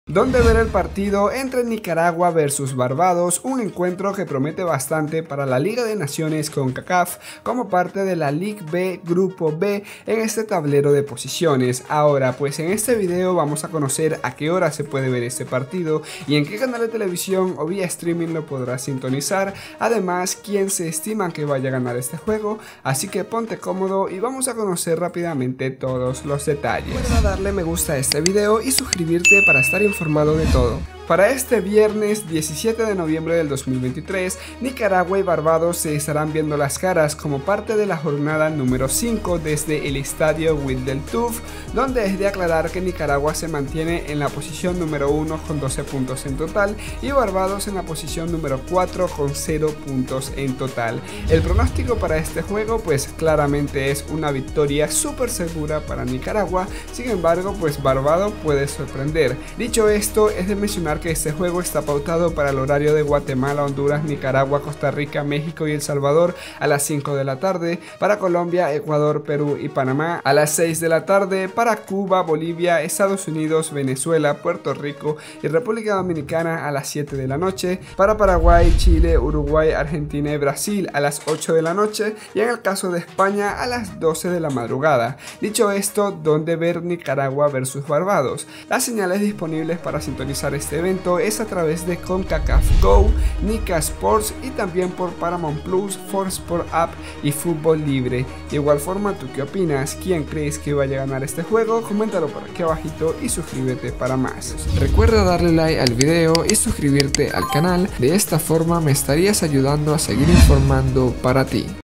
Nicaragua vs. Barbados en vivo, donde ver, a que hora juega nicaragua vs. barbados Concacaf 2023 hoy
Donde ver el partido entre el Nicaragua vs Barbados que está dando mucho que decir en las redes sociales como parte de Liga de Naciones Concacaf, un partidazo que se disputara en el Estadio wildey turf, para hoy viernes 17 noviembre 2023 y que indudablemente mantendrá a los hinchas expectantes, nicaragua vs. barbados un encuentro prometedor, cual es el pronostico del partido quien ganara o cuales son las predicciones / prediccion y además ¿En qué horario y en qué canal se puede estar viendo en vivo y en directo? Vamos a conocer todos los detalles.